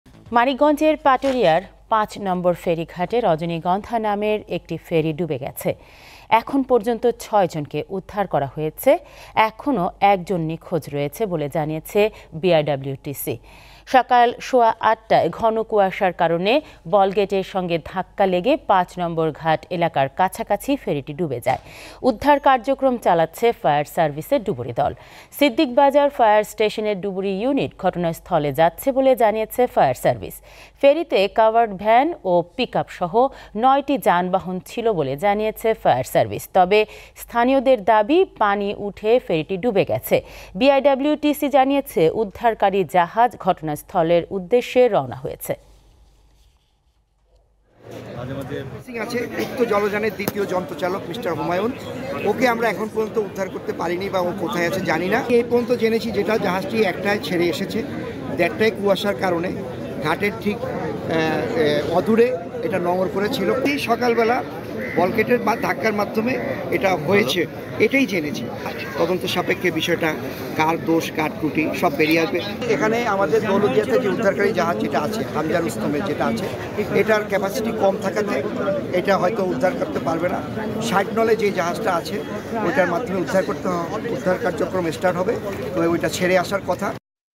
পাটুরিয়ায় ফেরিডু'বি; এখন পর্যন্ত উদ্ধার ৬ | Paturia | Ferry | DeeptoNews
পাটুরিয়ায় ফেরিডু'বি; এখন পর্যন্ত উদ্ধার ৬

Address:
Deepto TV
7/A/GA Tejgaon Industrial Area,